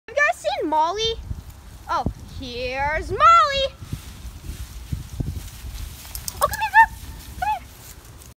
Here's Molly 😛
This probably took us 20 to 40 try's to complete Molly just kept moving  we had to be in there with her to hold her down lol 😂